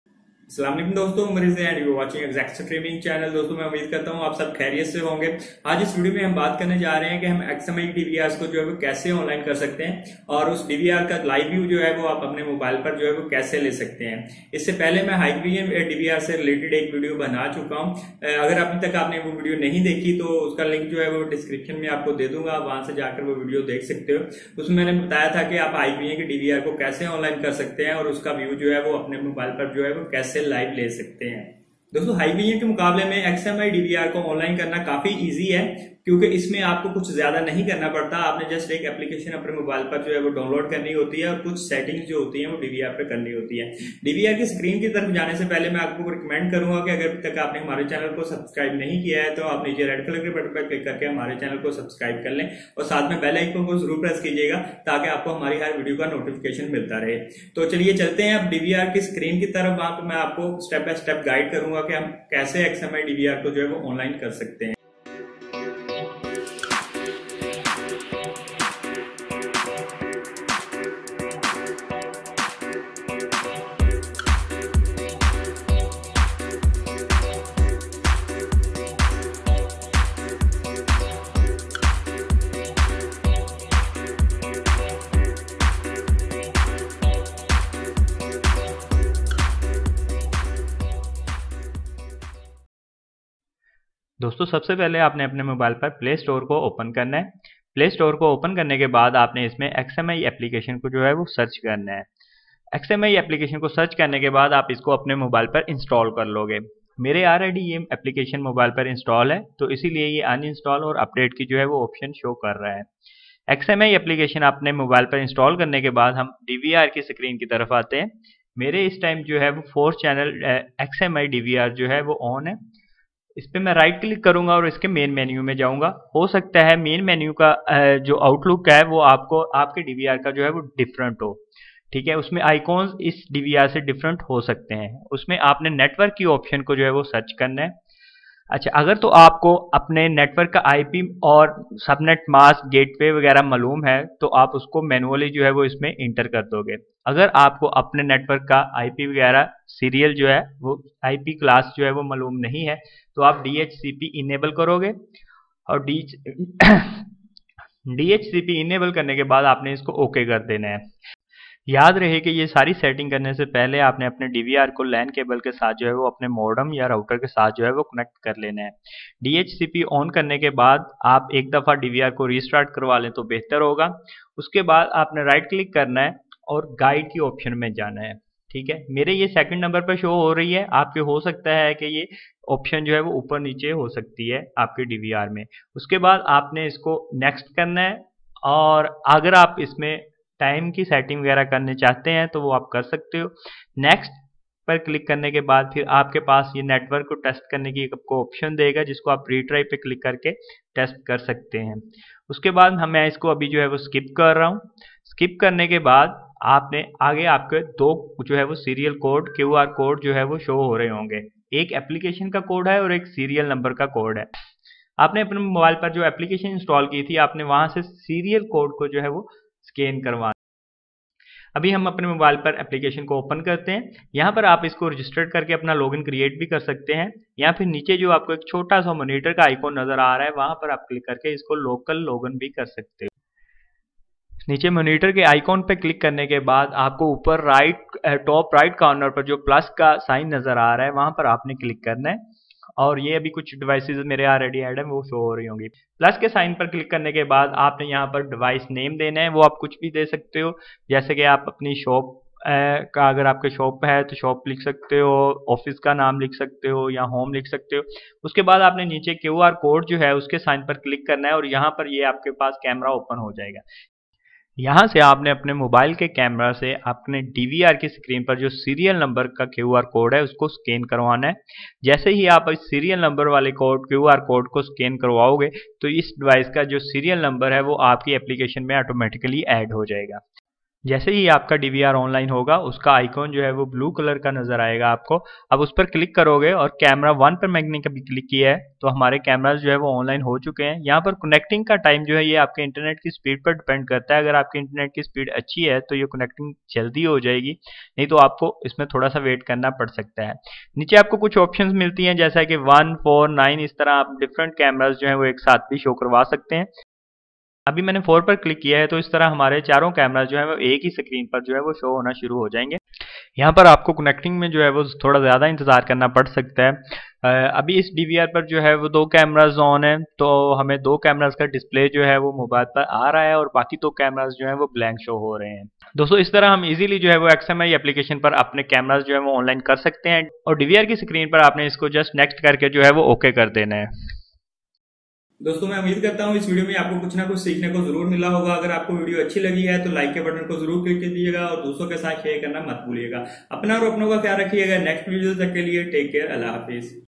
How to Online CCTV Cameras With Xmeye DVR - XmEye App
Hello friends in this video we will learn how we can online DVR with xmeye mobile application. XmEye App available for android and iOS. You need to install XmEye application on your mobile for CCTV cameras online view. Watch complete video step by step.